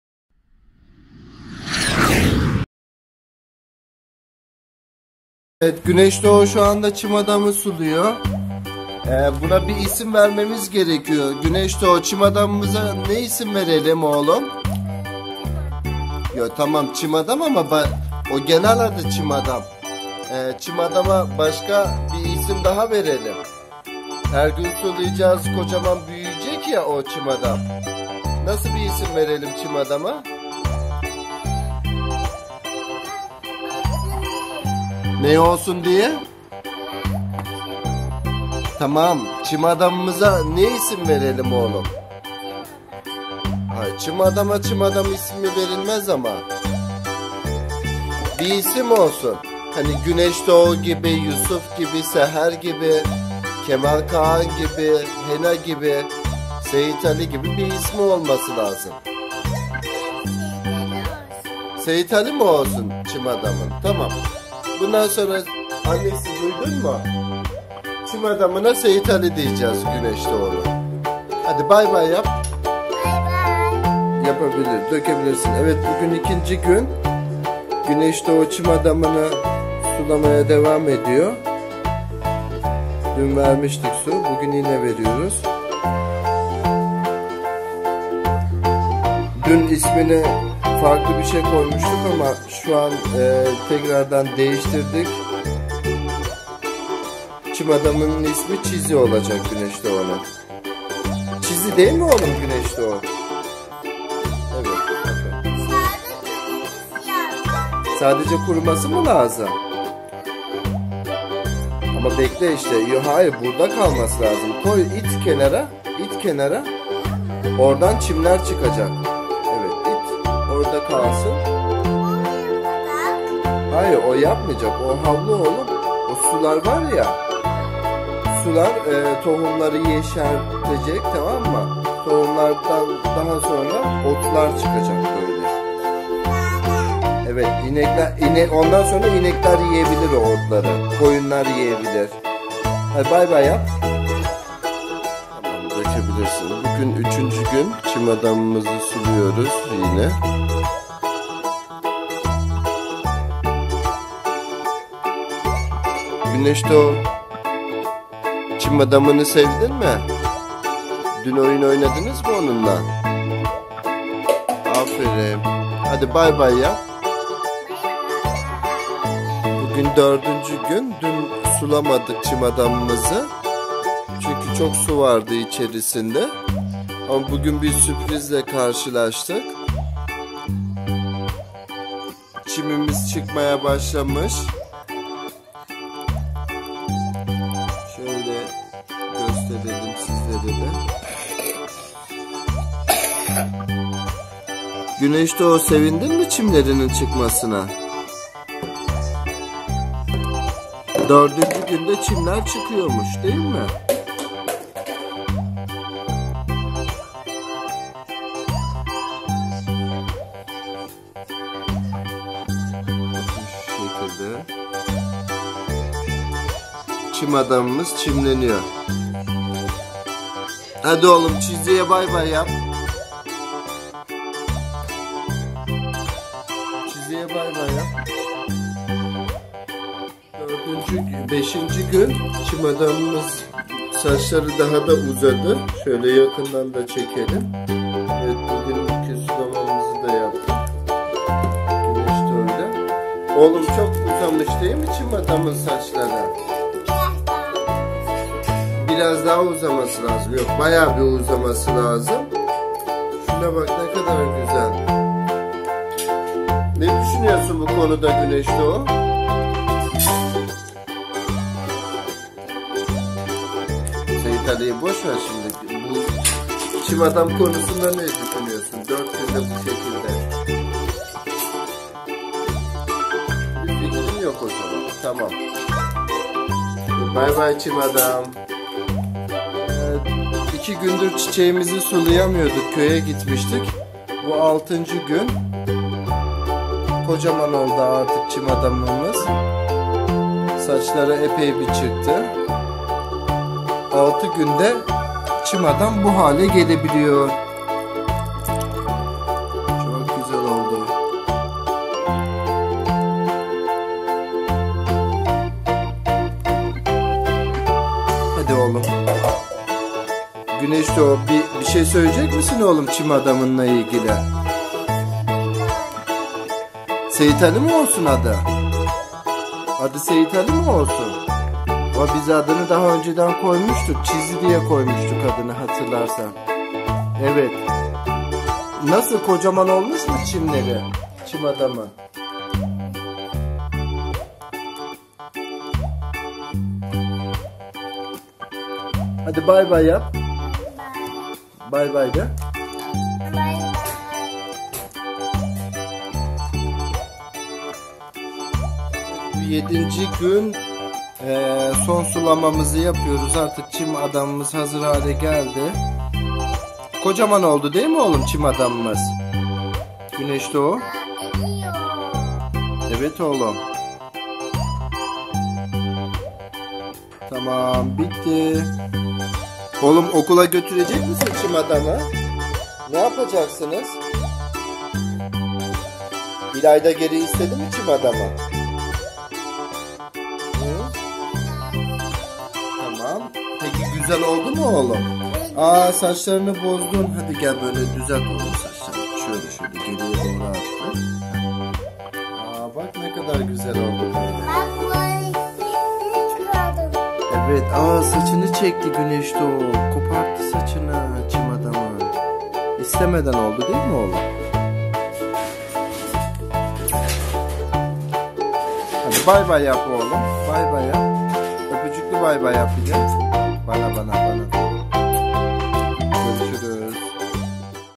Çim adam yaptık - eğlenceli çocuk oyunları
Kreşte yaptıkları çim adamı 7 günde sulayarak çimlendirdik.